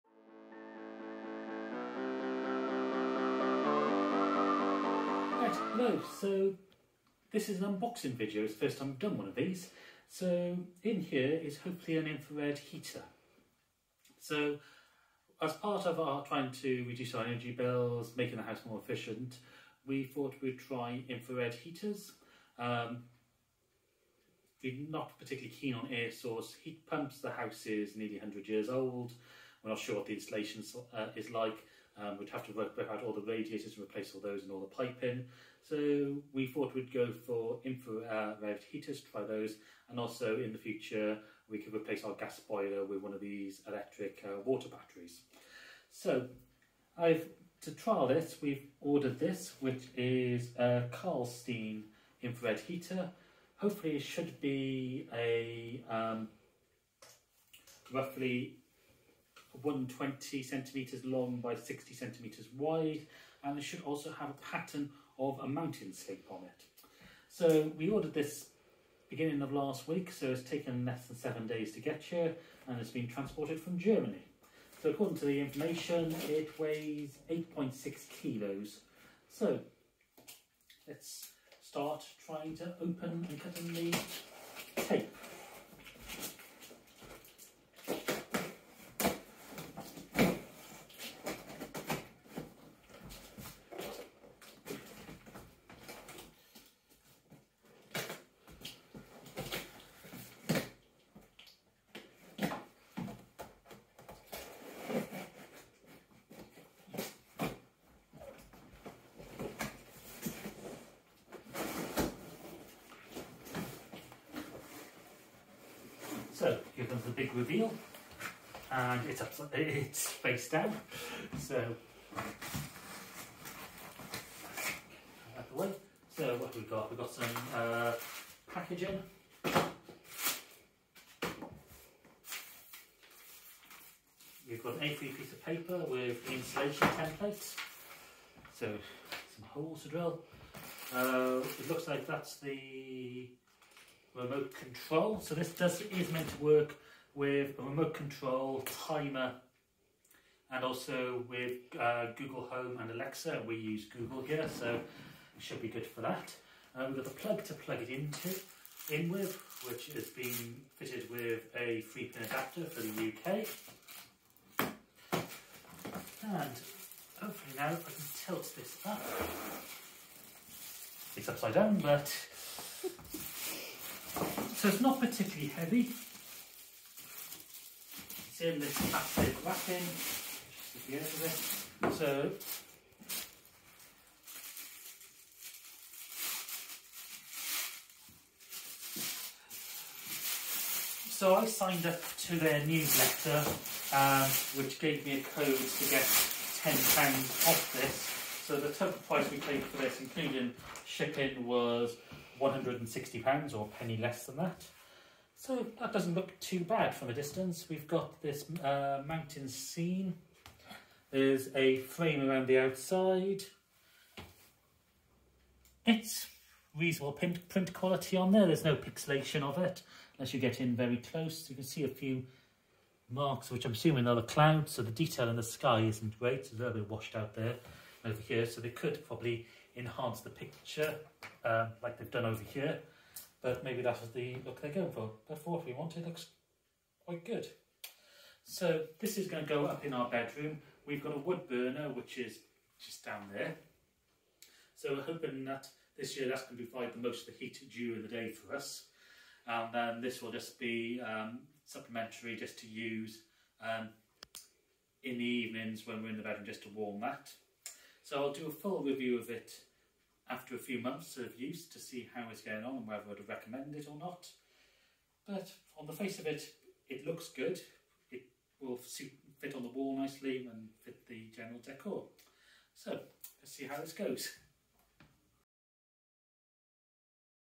Unboxing a Klarstein Wonderwall Air Art Smart Infrared Heater
As part of making the house more energy efficient and using the energy produced from the solar panels on the roof, I have decided to try an infrared heater.

In this video I unbox a  Klarstein Wonderwall Air Art Smart Infrared Heater (700W, 120 x 60 cm) in Mountains design.

This video is not sponsored and if you want to check out the heater, here is a link to it on the Klarstein website.